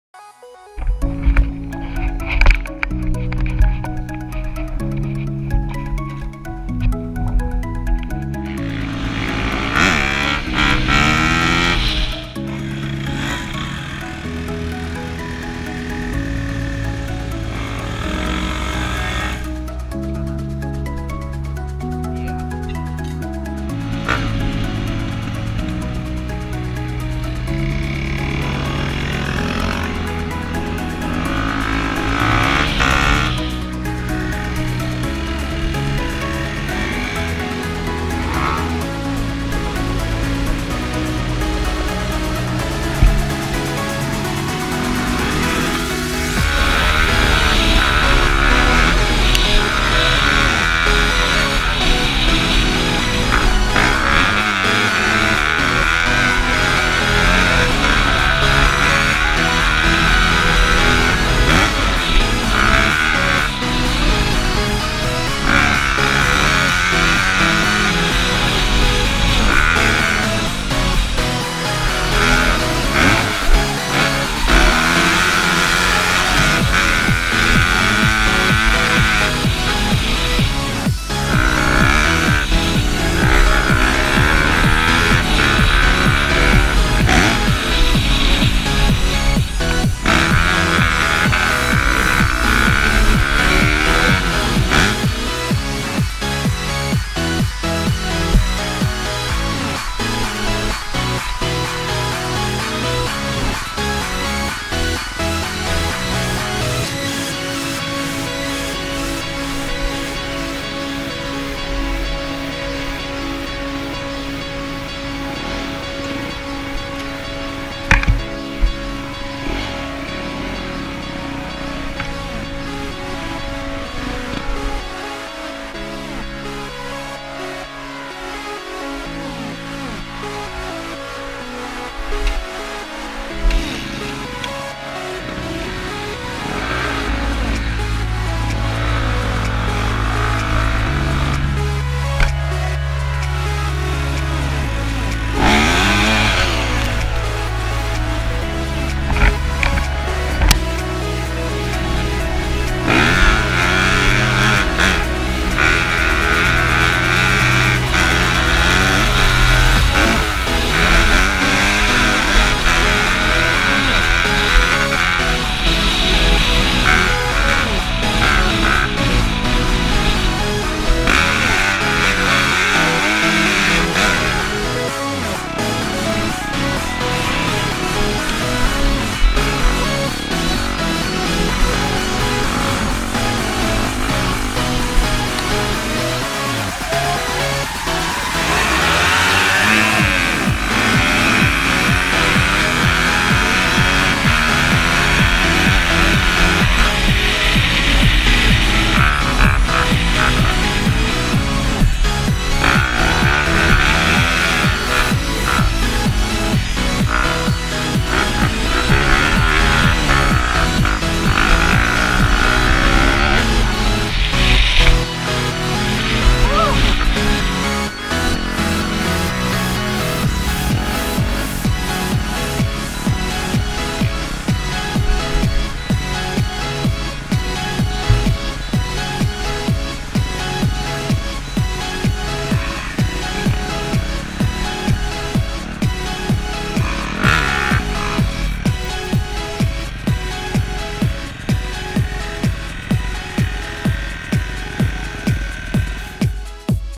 First Track Ride and Trail Ride With David on My 2016 YZ250F
2016 YZ250F
This was recorded at Birch Creek and a place down from my house